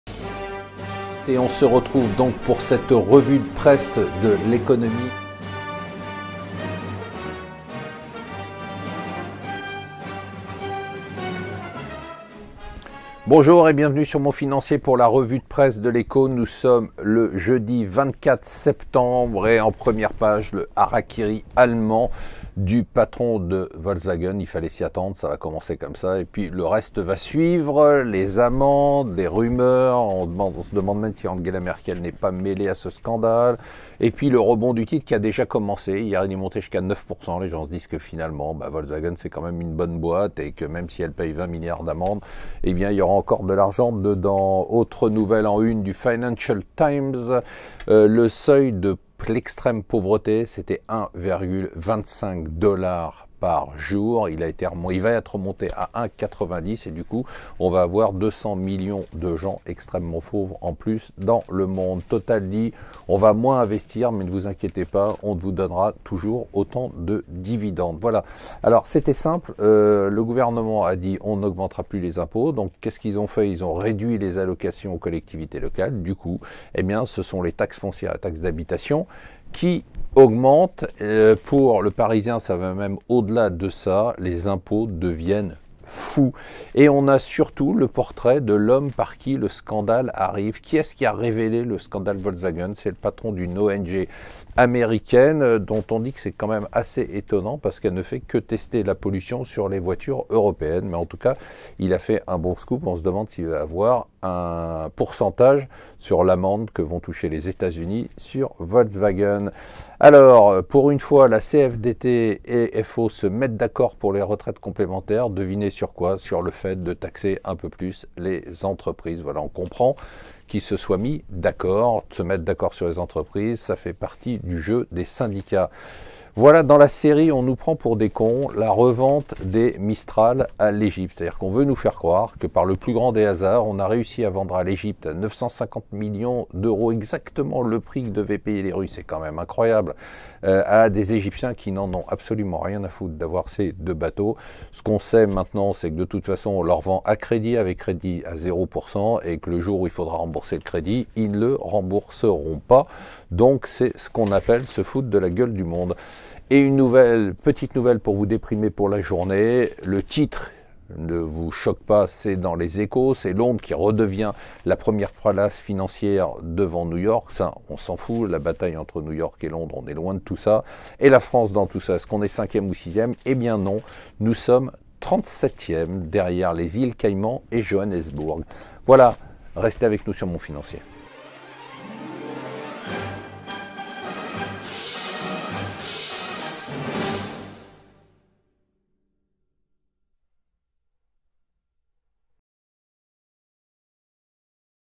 La cure de désintoxication
La cure de désintoxication, Le retour à l'économie préhistorique, L'arnaque des Mistal, Hara Kiri en Allemand, Paris 37ème place financière